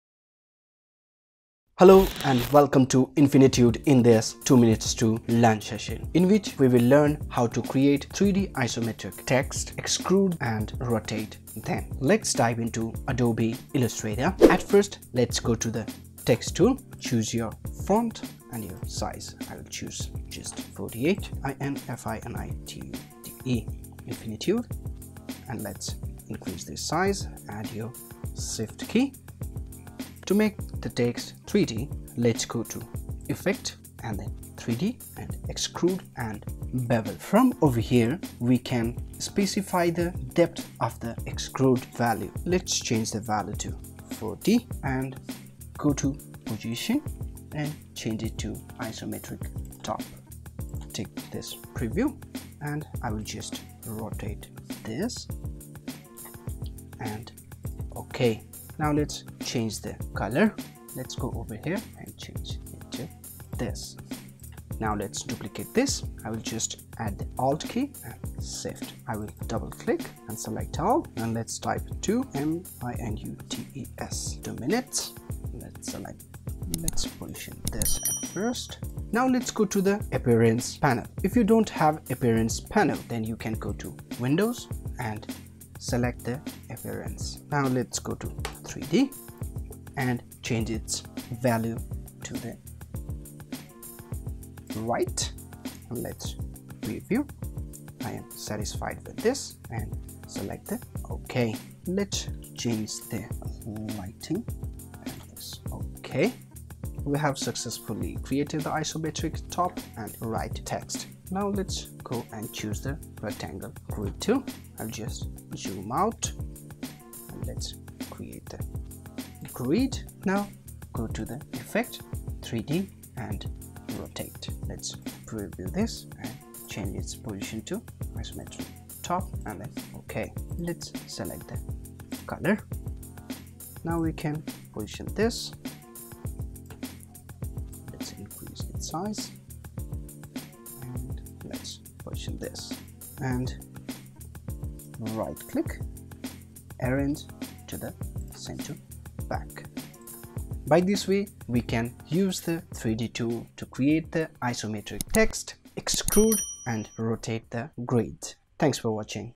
How to Create 3D Isometric Text | Extrude & Rotate | Illustrator Tutorials
This Adobe Illustrator Tutorial video will let you know how to create 3D Isometric Text, Extrude the Isometric Text, Rotate the Rectangle Grid using the 3D Tool of Adobe Illustrator.